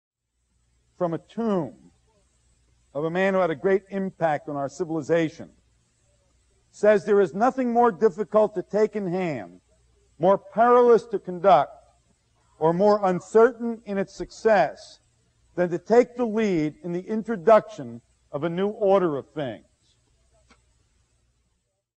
A New Order of Things - Werner Erhard Quote
There is nothing more difficult to take in hand, more perilous to conduct, or more uncertain in its success, than to take the lead in the introduction of a new order of things.

All great things began as an idea, that once seemed impossible.

- Niccolo Machiavelli, The Prince (I recommend actually reading his books)